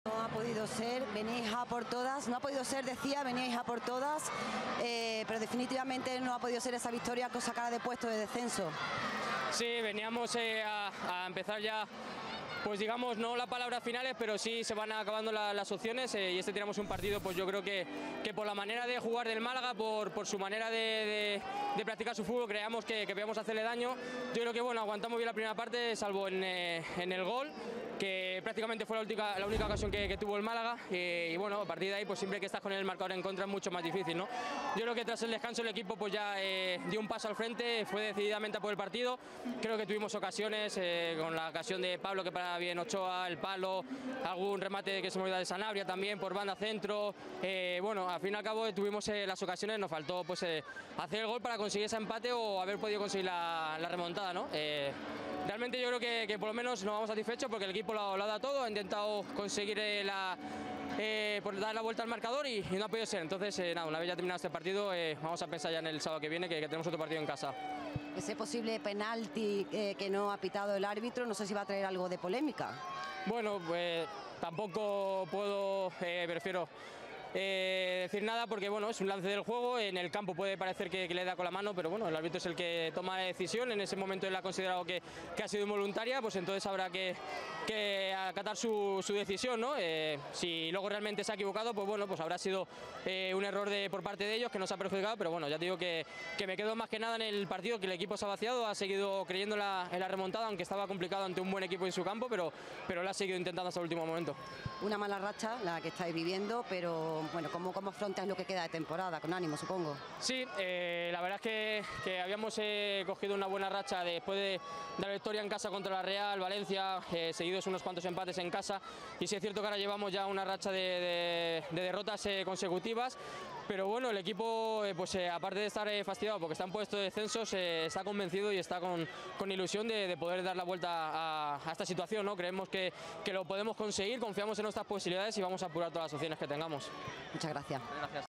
Lora: "Nos vamos satisfechos porque el equipo lo ha dado todo"
Entrevista a Lora tras el Málaga CF (1-0) Sporting de Gijón Suscríbete al canal oficial de LaLiga en HD | Subscribe to the Official Channel of LaLiga in High Definition | Lora tras el Málaga CF (1-0) Sporting de Gijón J29 Liga BBVA 2015/2016